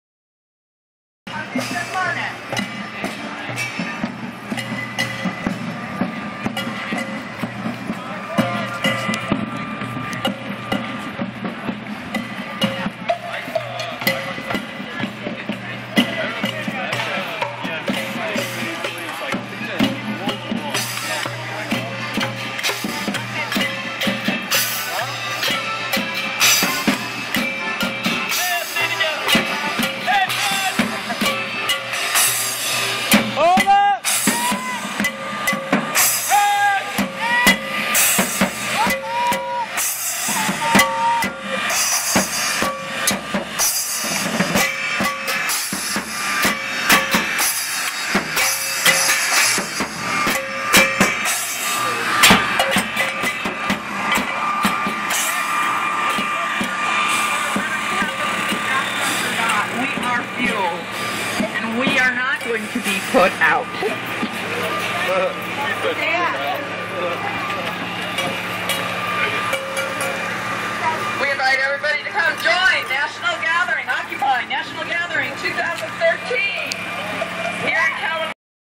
2nd Occupy National Gathering (Aug. 21st, 2013)
Marching downtown Kalamazoo.